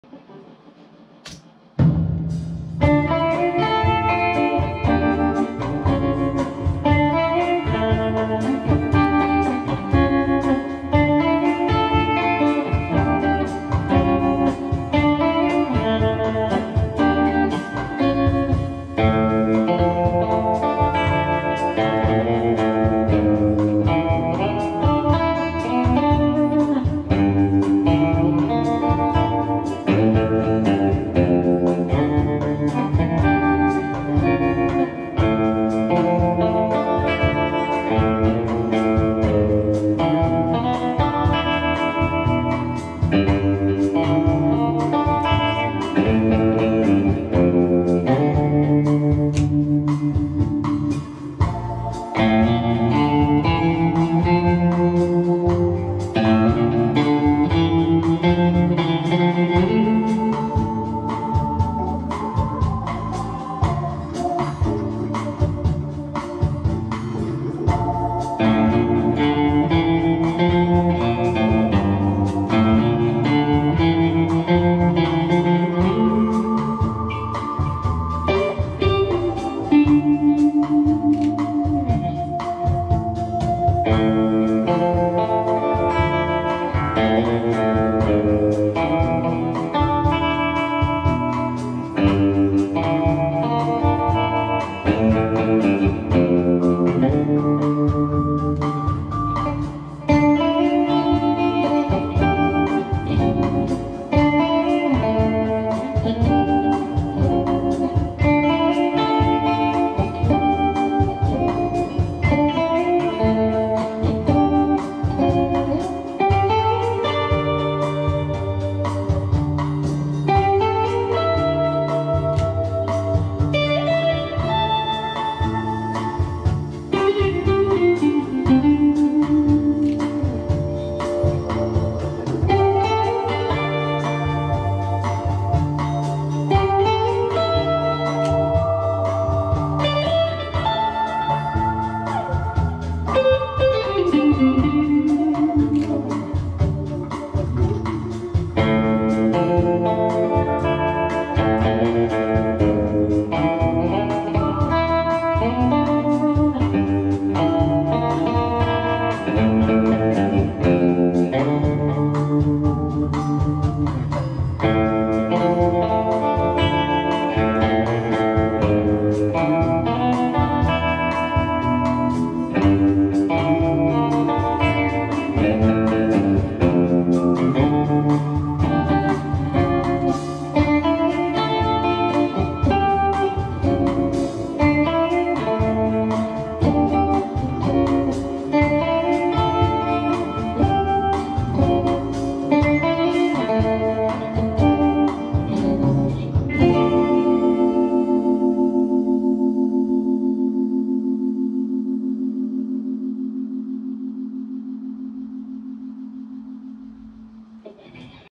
Mysterium Steve Whitton plays his 1st Kenny Vaughn song with a Tom Anderson guitar
Lately, I have been obsessed with Kenny Vaughn from Marty Stuart's
band - The Superlatives.

Kenny is a top Nashville player